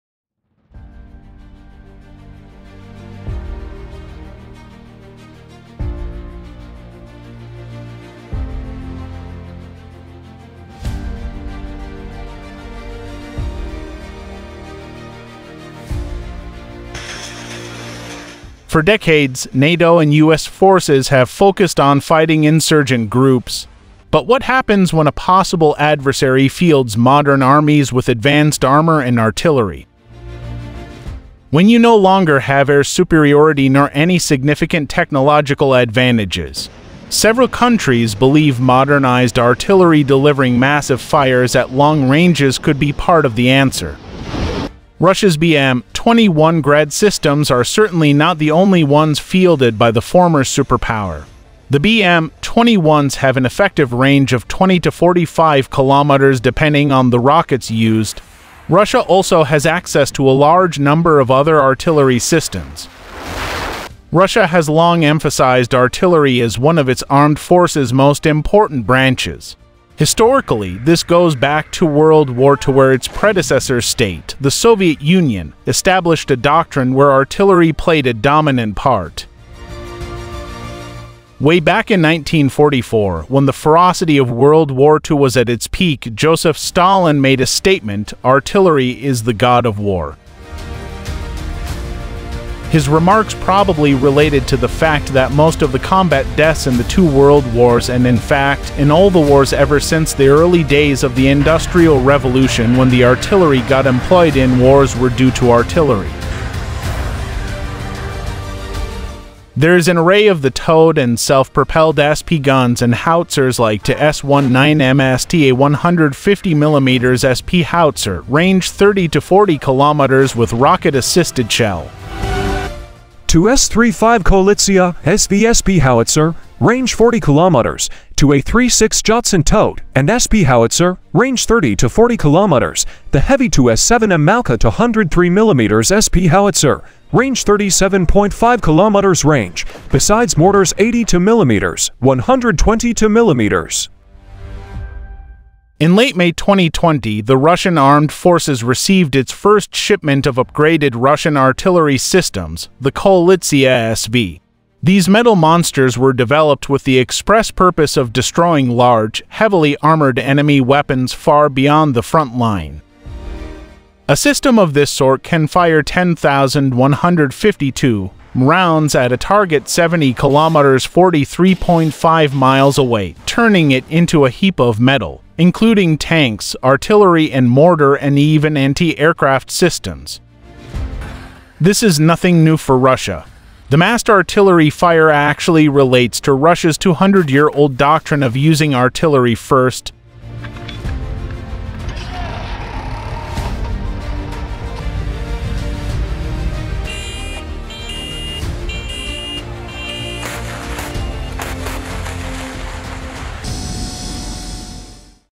Revealed: Here's Russia'n Its New Howitzer You've Never Seen Before
For decades, NATO and US forces have focused on fighting insurgent groups. But what happens when a possible adversary fields modern armies with advanced armor and artillery?

When you no longer have air superiority nor any significant technological advantages? Several countries believe modernized artillery delivering massive fires at long ranges could be part of the answer.

Russia’s BM-21 Grad systems are certainly not the only ones fielded by the former superpower. The BM-21s have an effective range of 20-45kms (depending on the rockets used). Russia also has access to a large number of other artillery systems.